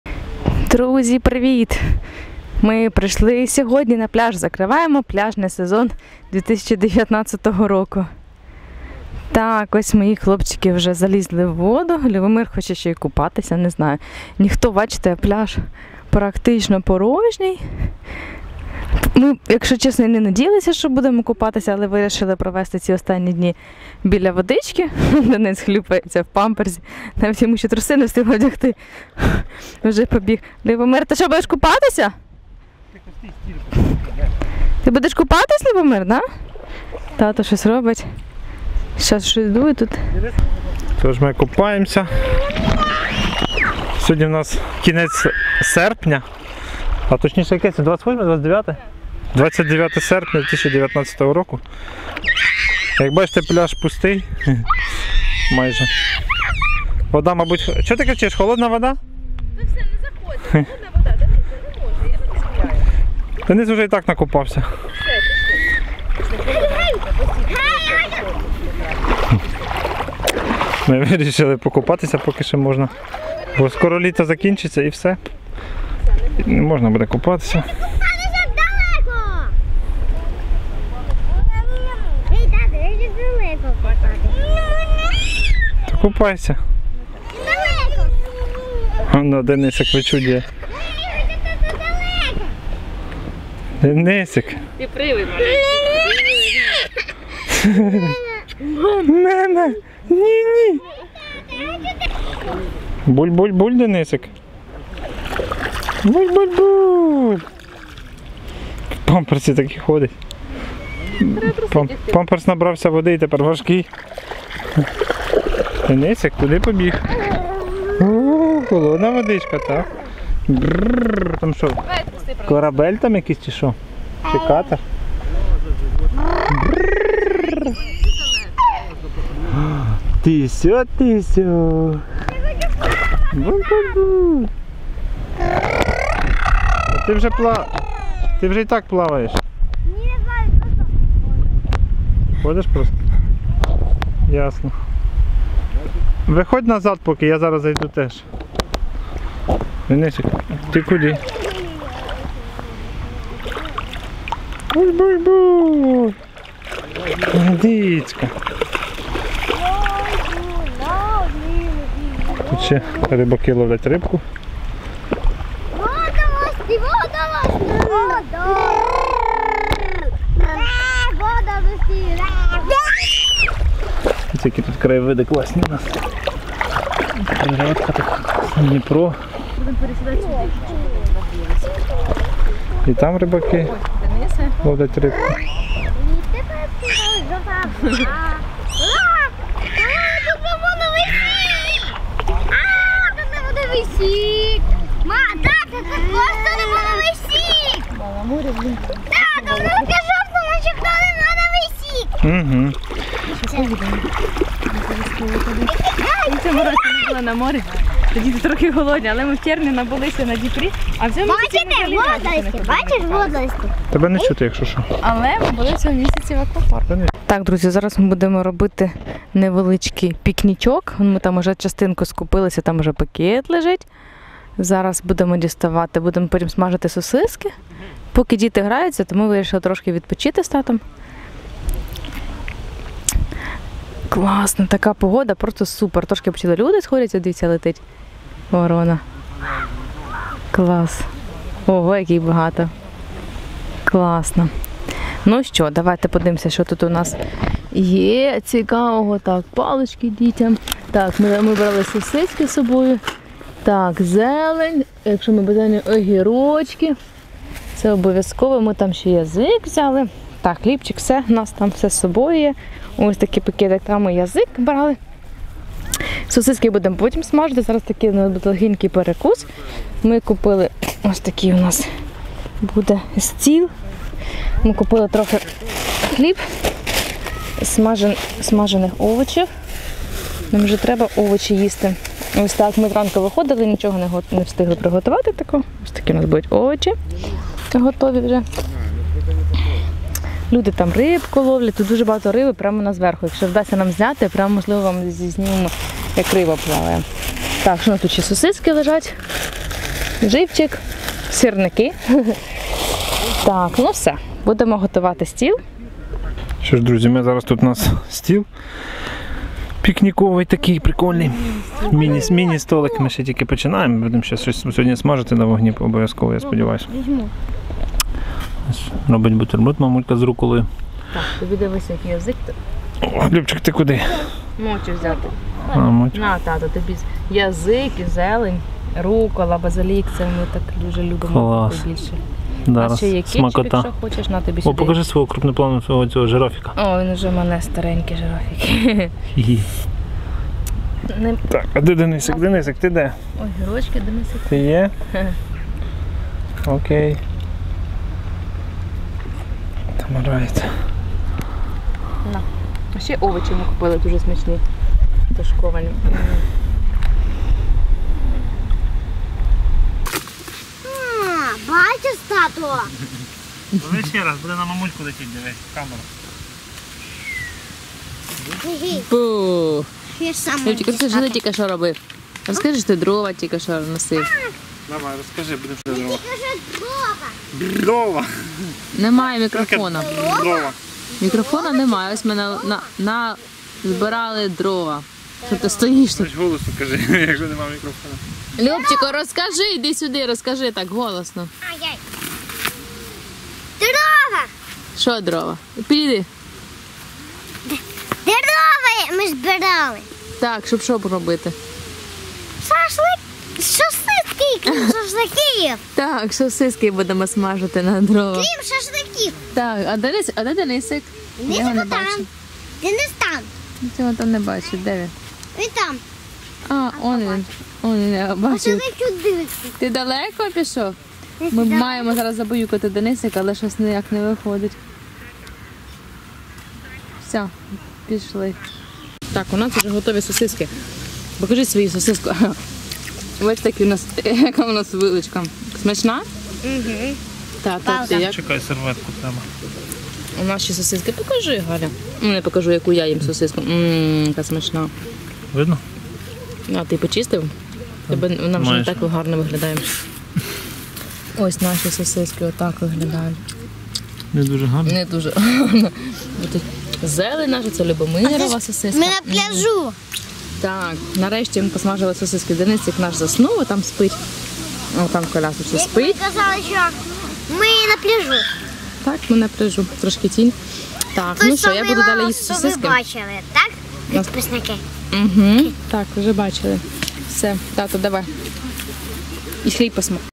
VLOG Закриття пляжного сезону 2019 | ВЛОГ Закрытие пляжного сезона 2019 | Babasiky
Влог про те, як Бабасіки закривали пляжний сезон 2019 року в кінці серпня. Було ще тепло і ми купалися, проте людей на пляжі було вже мало.
Влог о том, как Бабасики закрывали пляжный сезон 2019 года в конце августа. Было еще тепло и мы купались, однако людей на пляже было уже мало.